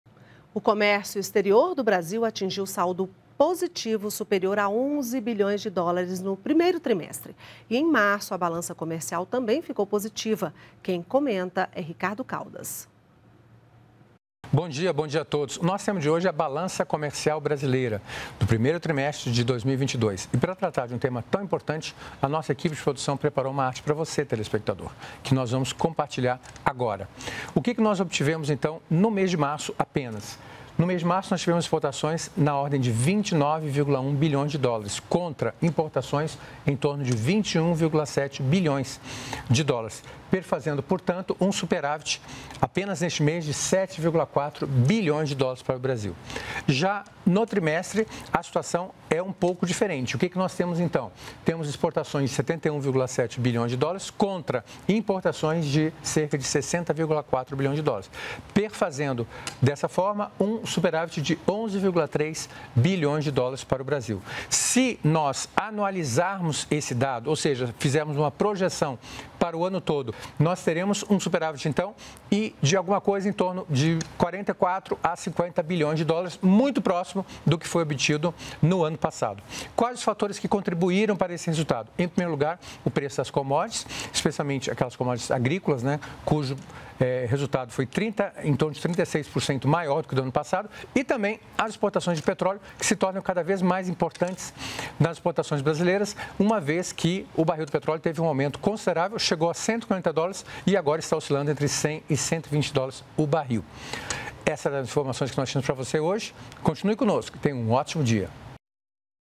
Economia | Comentário Ricardo Caldas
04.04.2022 - O comércio exterior brasileiro atingiu saldo positivo superior a R$ 11 bilhões no primeiro trimestre. E em março, a balança comercial também ficou positiva. Quem comenta os dados é o cientista político Ricardo Caldas.